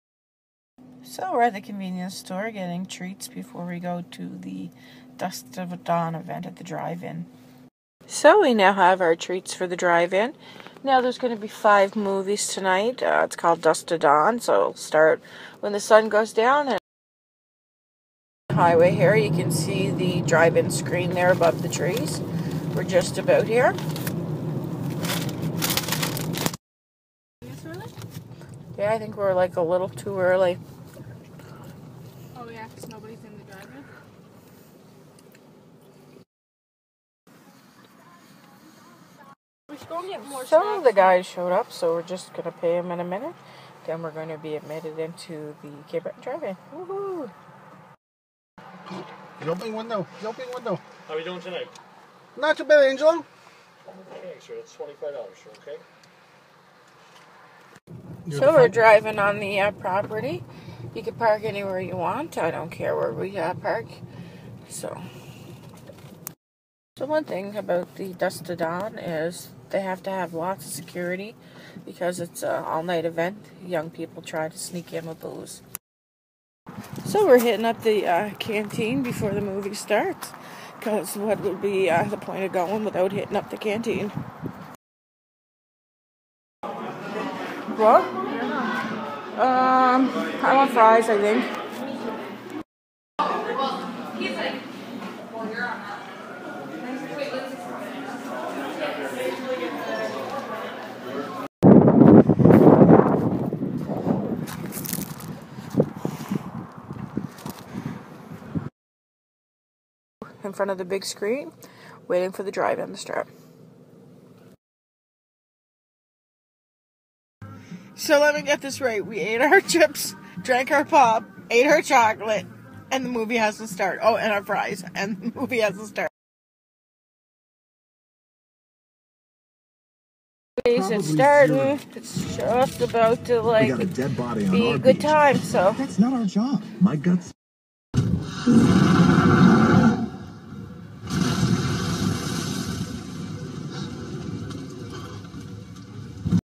Checking out the Cape Breton Drive In May 22, 2017
If you have ever spent any time in Cape Breton chances are you have went to the Cape Breton Drive in to watch a movie with either friends or family